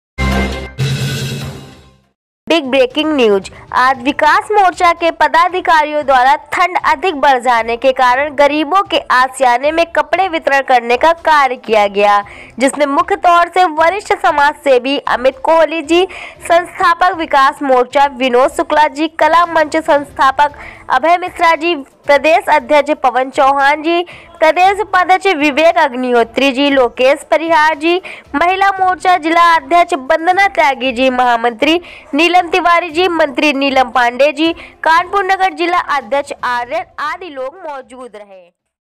विकास मोर्चा के पदाधिकारियों द्वारा गरीबों के आशियाने में कपड़े वितरण किये गए
जिसमें मुख्य तौर से वरिष्ठ समाजसेवी अमित कोहली जी संस्थापक विकास मोर्चा विनोद शुक्ला जी कला मंच संस्थापक अभय मिश्रा जी प्रदेश अध्यक्ष पवन चौहान जी प्रदेश उपाध्यक्ष विवेक अग्निहोत्री जी लोकेश परिहार जी महिला मोर्चा जिला अध्यक्ष बंदना त्यागी जी महामंत्री नीलम तिवारी जी मंत्री नीलम पांडे जी कानपुर नगर जिला अध्यक्ष आर्यन आदि लोग मौजूद रहे